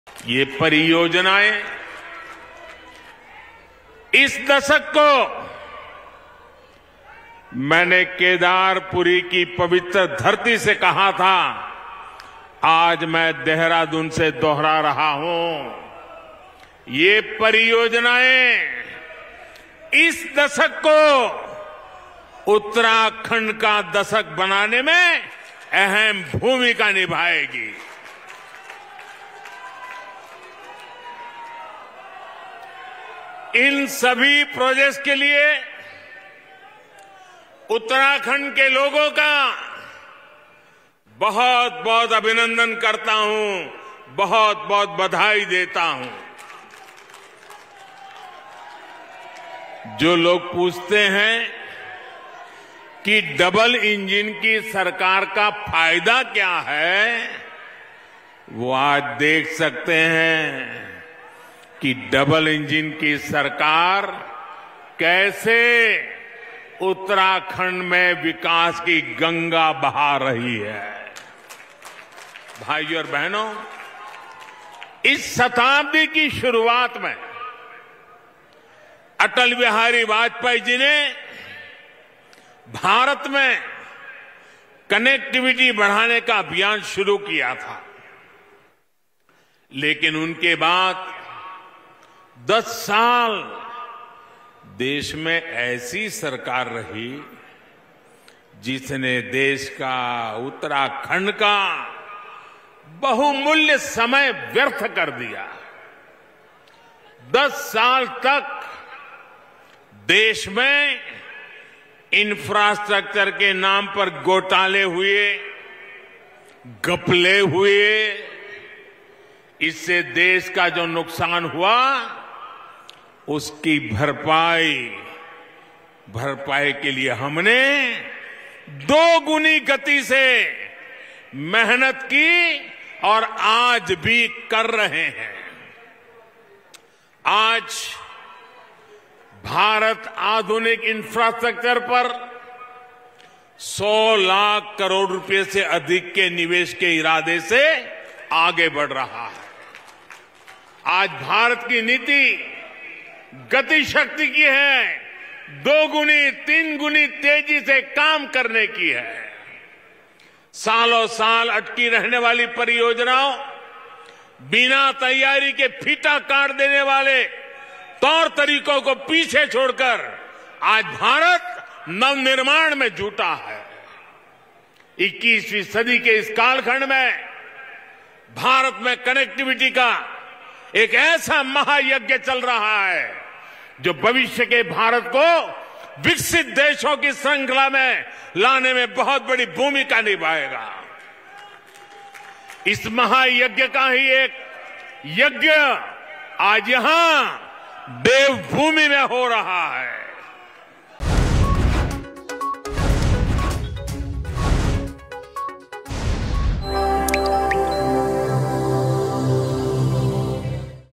ଦେହରାଡୁନରେ ବିକାଶମୂଳକ ପ୍ରକଳ୍ପର ଶୁଭାରମ୍ଭ କଲେ ମୋଦୀ
ଉତ୍ତରପ୍ରଦେଶର ଦେହରାଡୁନରେ ବିକାଶମୂଳକ ପ୍ରକଳ୍ପର ଶୁଭାରମ୍ଭ କରିଛନ୍ତି ପ୍ରଧାନମନ୍ତ୍ରୀ ନରେନ୍ଦ୍ର ମୋଦୀ ।